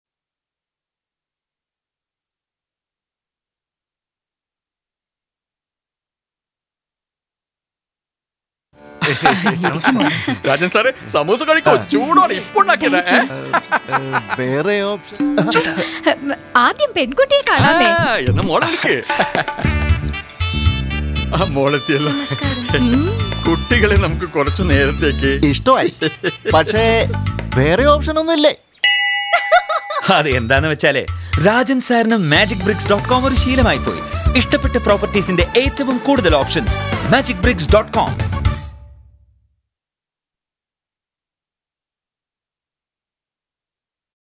Magicbricks TV ad in Malyalam
Rajan is a self-made man; confident of what he wants and not hesitant to speak his mind. Our man is obsessed with options. He is used to getting the maximum options of the best properties on MagicBricks.com. This is the central idea behind the first film, where Rajan with his wife and younger brother goes to a prospective bride's house.  His wife and brother are sold on the girl and are convinced that she is the one. That's when the penny dropping moment comes as Rajan nonchalantly asks for more options!